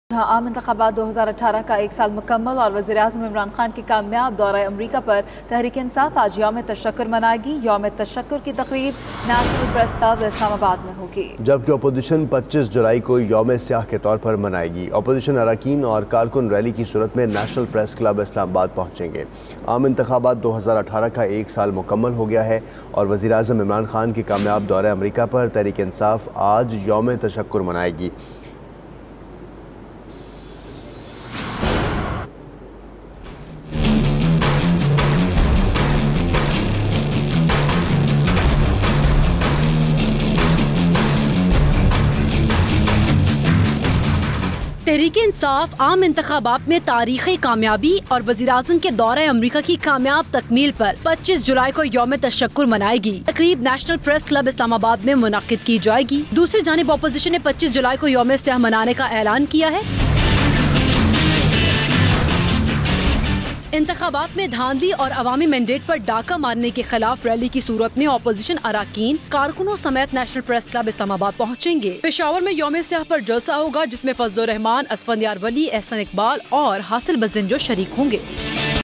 PTI to give thanks, while opposition observes anniversary of 2018 elections as ‘black day’
Watch:  PTI to give thanks, while opposition observes anniversary of 2018 elections as ‘black day’ | 25 July 2019 | 92NewsHDUK